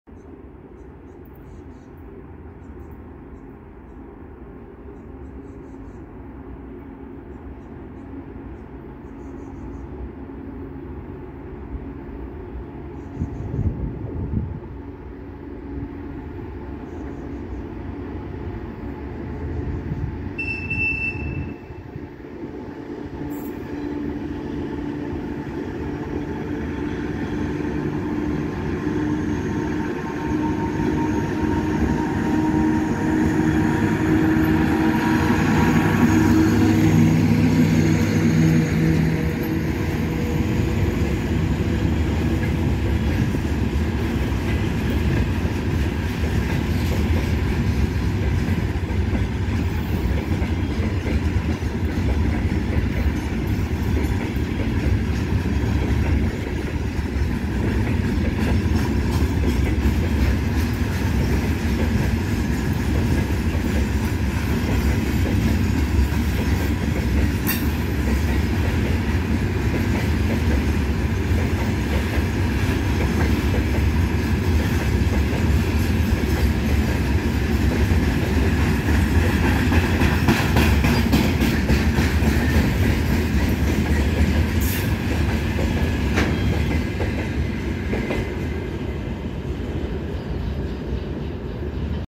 Fenniarailin juna Heinolasta 26.6.2023
T 7157 (Heinola - Lauritsala) lähtee Lahdesta. Dr18 103 ja 28 Viroon rekisteröityä pankkovaunua kuormassa. Kaikki vaunut mallia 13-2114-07, paitsi kolme: yksi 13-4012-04, yksi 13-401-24 ja yksi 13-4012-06.